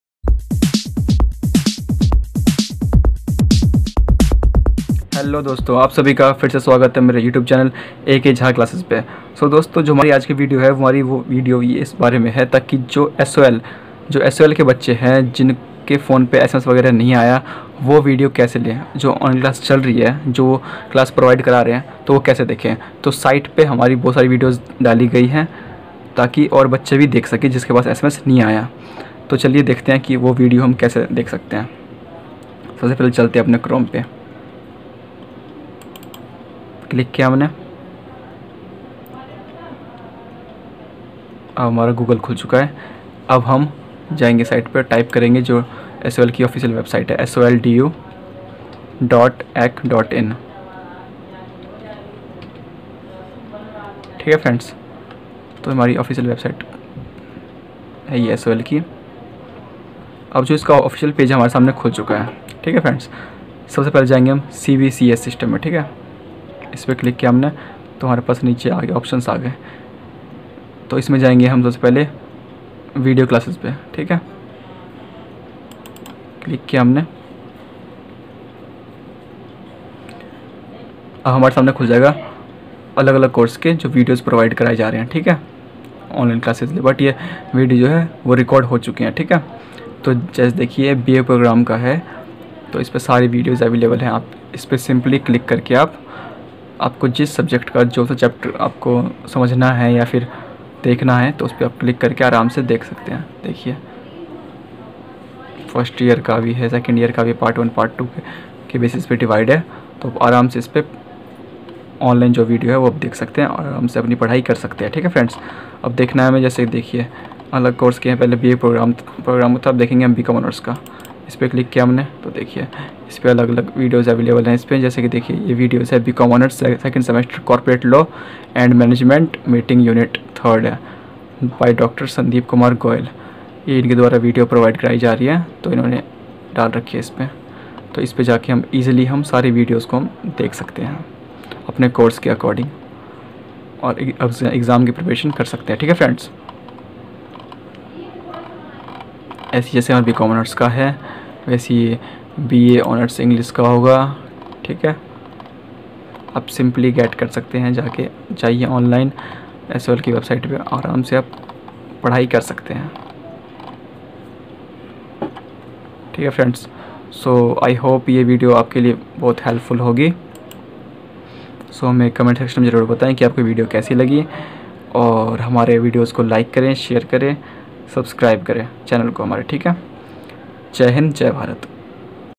How to get sol online class 2020 without getting SMS
Hello friends,this video will help you to get sol online class 2020.I hope those students didn't get sms from sol and face difficulties to get online class after this video you all will be able to get sol online classes 2020.